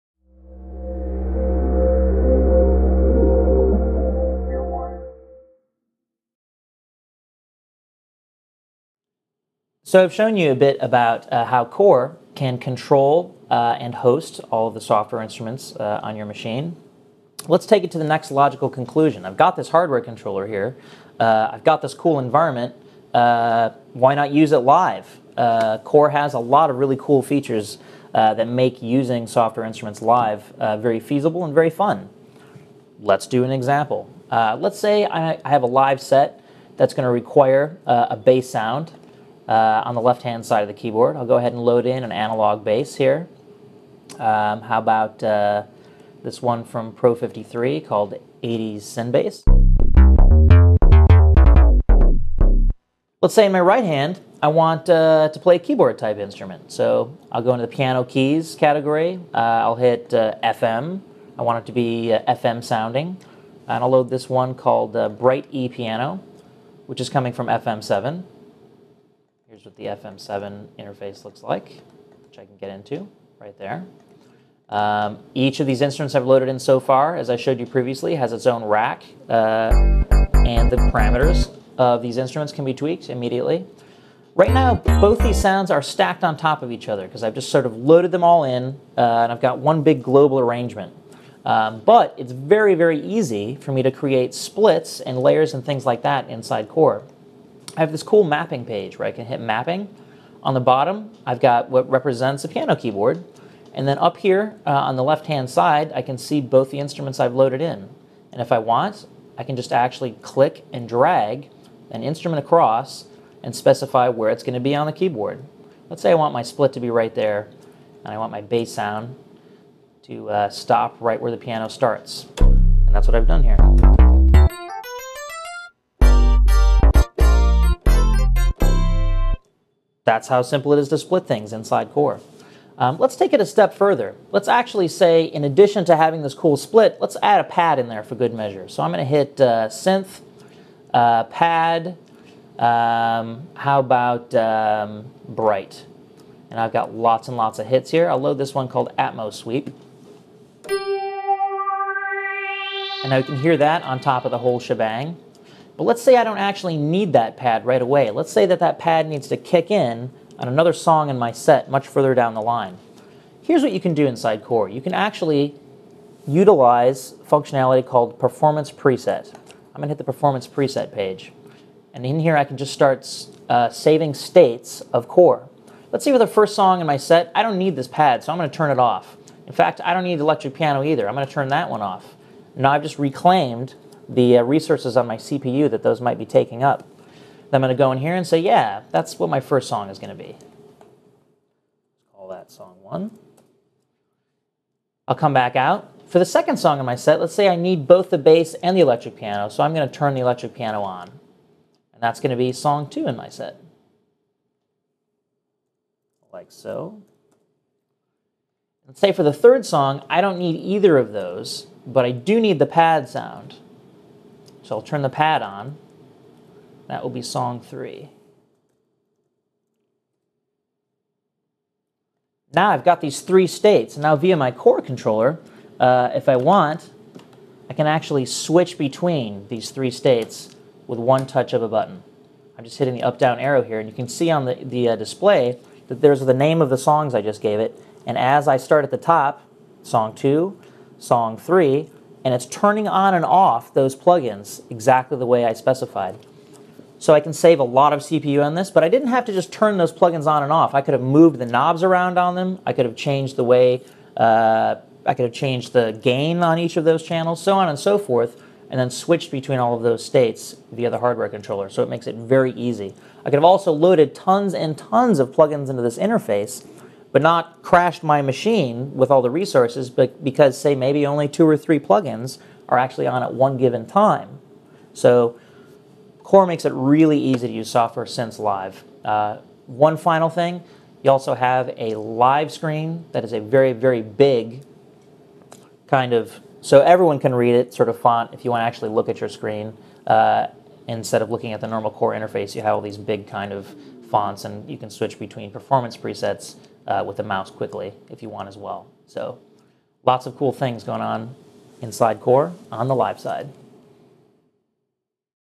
Native Instruments KORE: Live Applications
Josh from Native Instruments shows us how to set up presets that can quickly quickly be selected for live use, without bogging down your laptop with a lot of plugins operating at once.

Features

Third-party plugin support

24-bit/96 kHz USB interface

Two inputs and two outputs

MIDI in and out

Touch-sensitive, assignable knobs